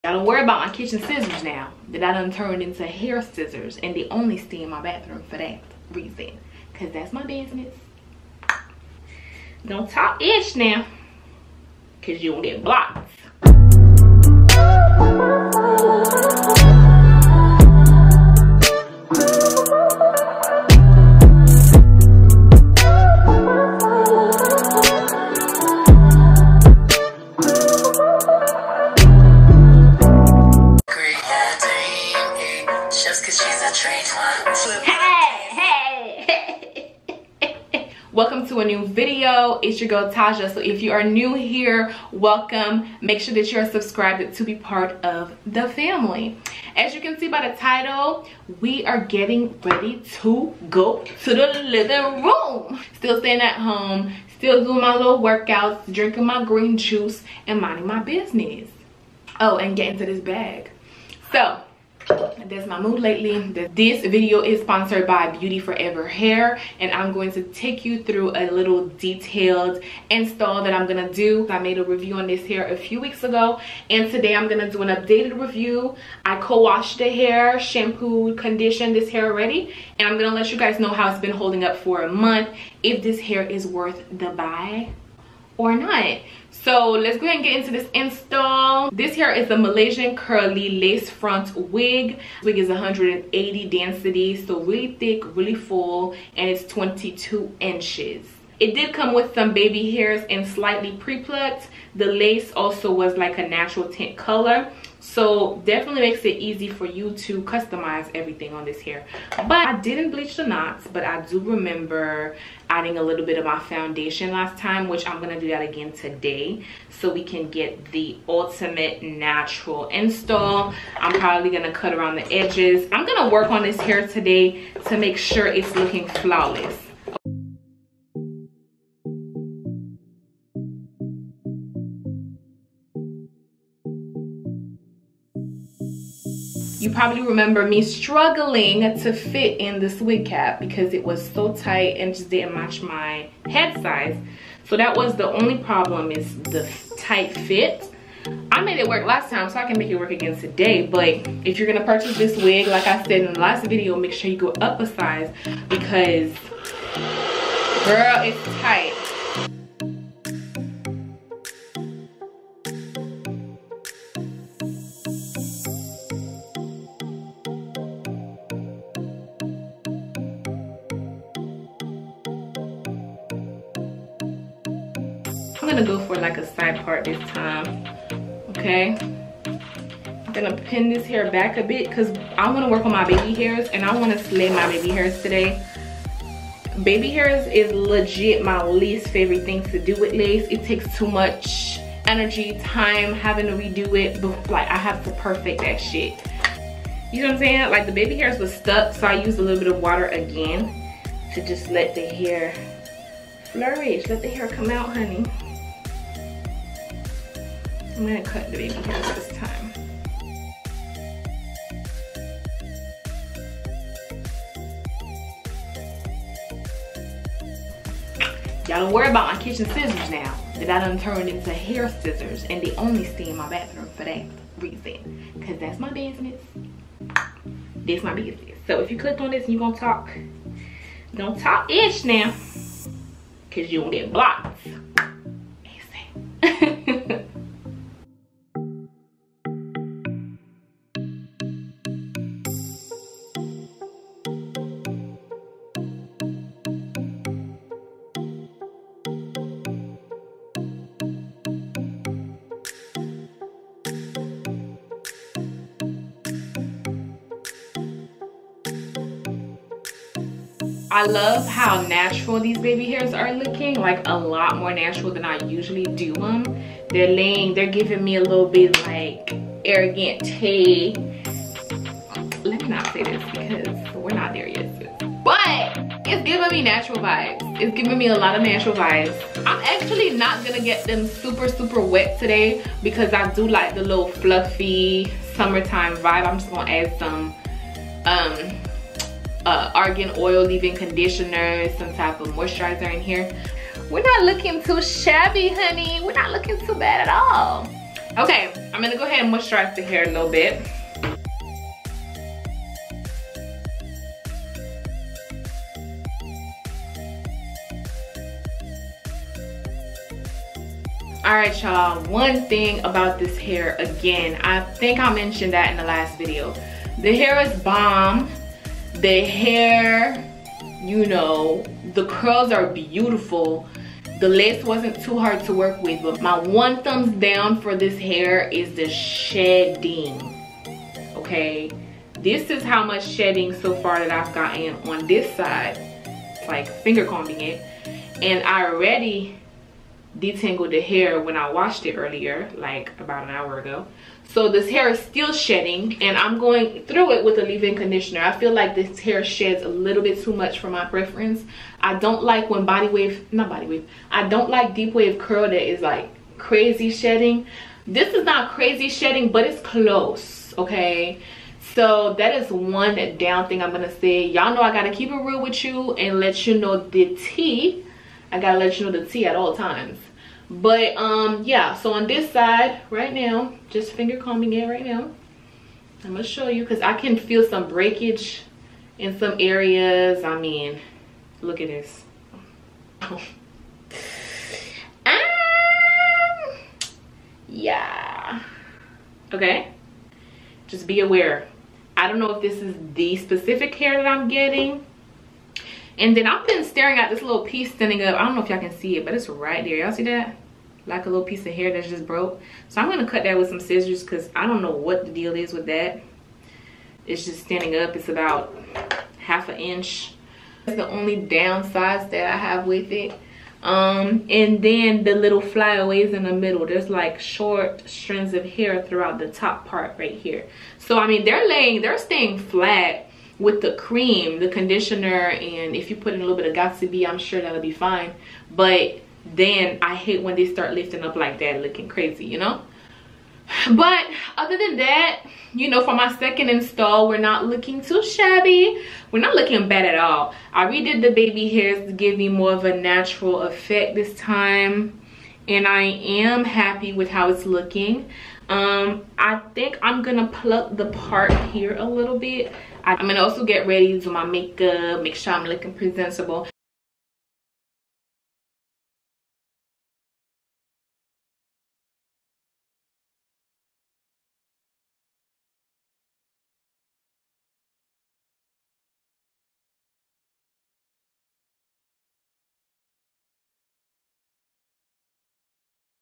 BEAUTYFOREVER HAIR 1 MONTH UPDATE ! IS IT WORTH IT? MALAYSIAN CURLY HAIR
Hey guys ! Y'all continue to be safe , I LOVE YALL! XO
Follow my business page : @tajahsluxboutique

Overall, I love the curl pattern of this hair , it’s thick and screams vacation !!! I recommend this hair , just be aware of shedding and order up a size !

Hair Details: Malaysian curly wig in 180% density 22 inches

$$$5 can Customize A Wig! Amazing!!

New!!! Arrival ombre and 99J Wigs
Amazon Beauty Forever Hair Store